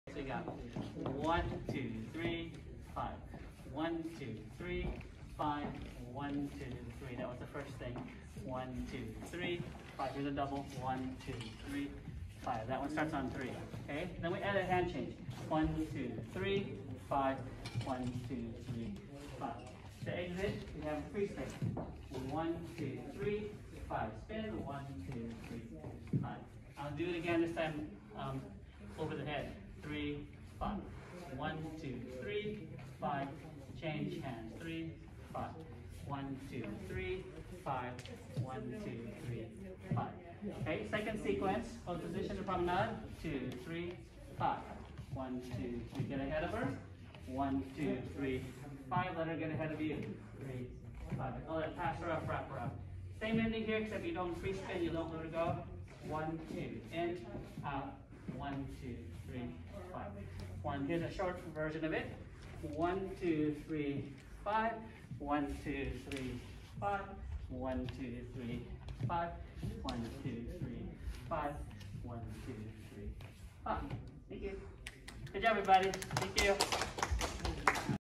03/18/2022 - Country 2step - basic 123-5-, single & double turn, shadow walk, etc
1. Basic 123-5-
2. single turn
3. double turn
4. shadow walk, roll her out
5. prom, natural, roll her in, walk, roll her out.